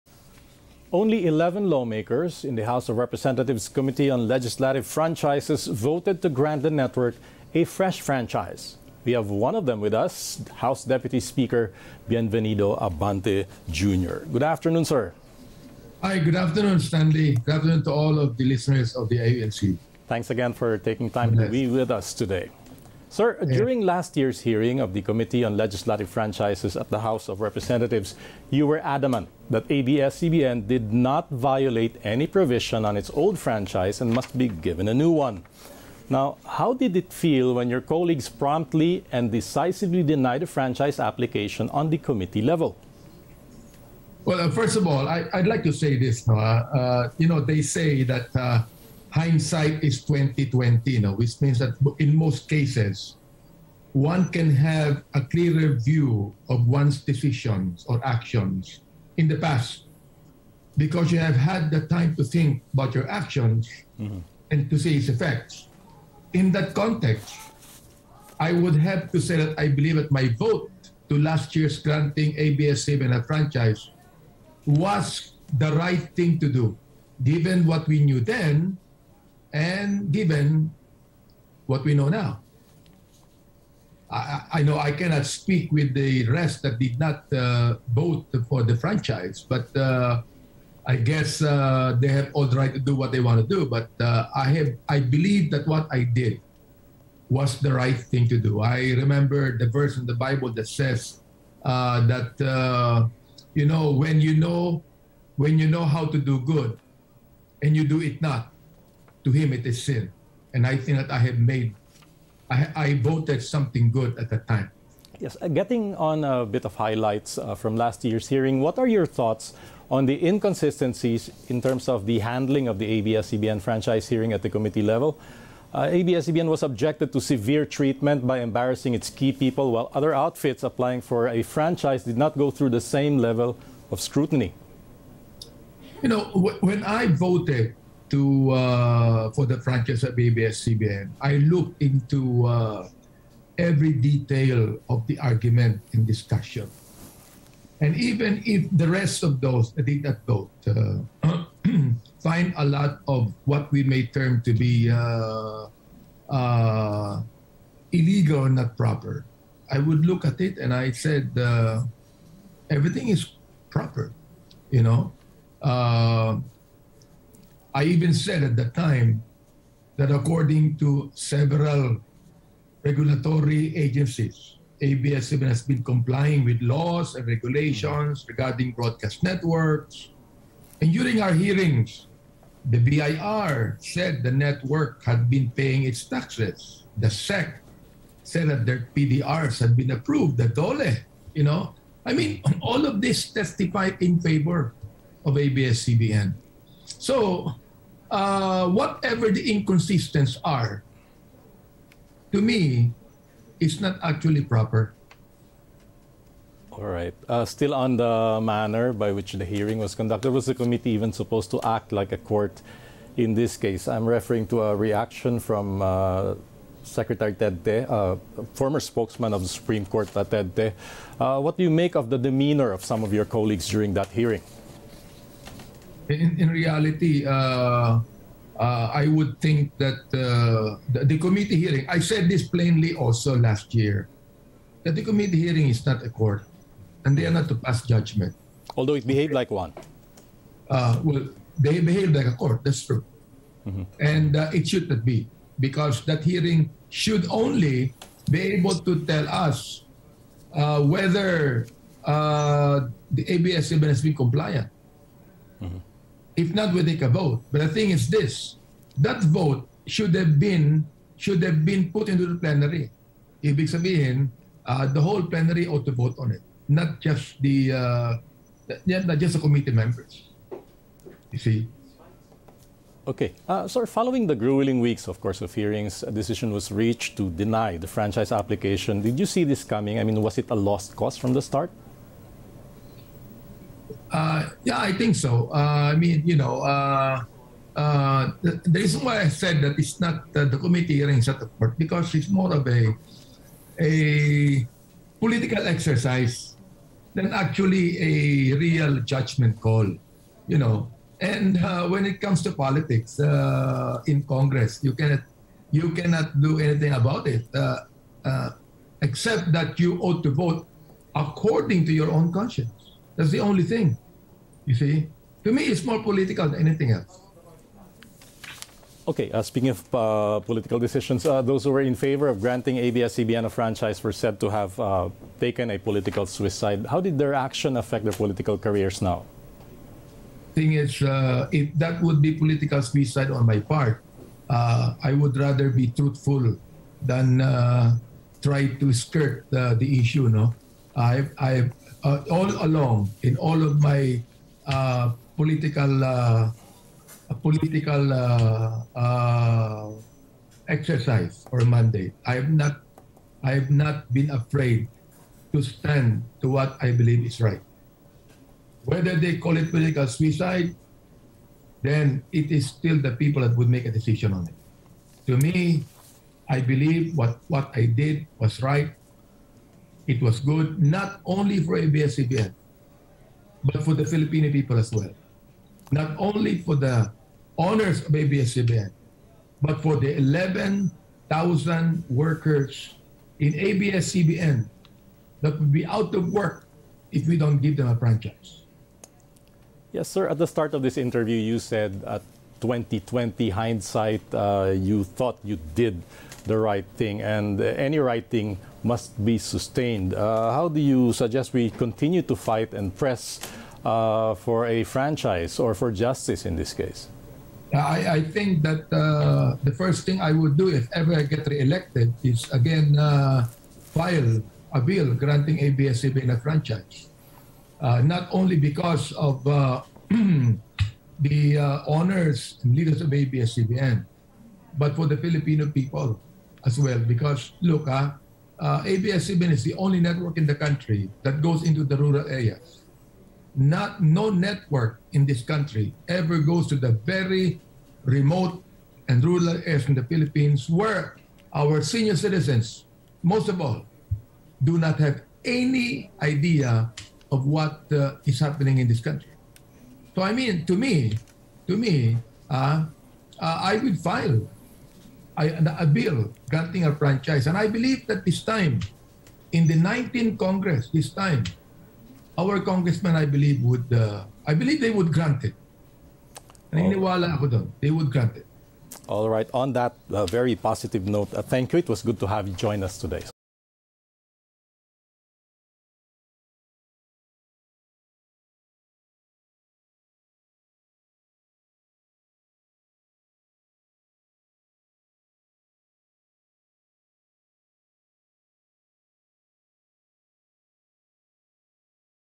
PH House Deputy Speaker to file bill seeking fresh franchise for ABS-CBN in next congress | ANC
Philippine House Deputy Speaker Bienvenido 'benny' Abante plans to file a bill granting media giant ABS-CBN a broadcast franchise in the next Congress.